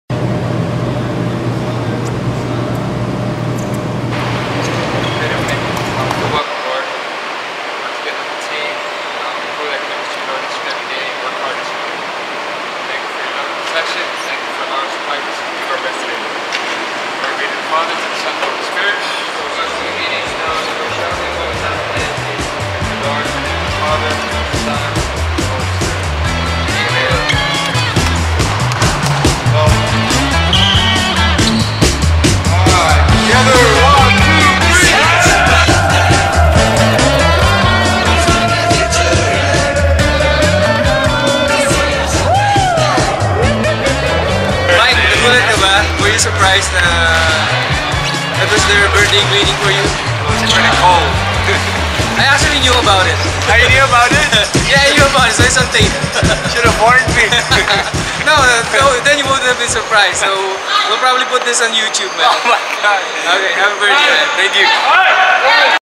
Alaska scandal with Mike Burtscher
Wet and wild. Mike Burtscher takes a shower after Alaska practice.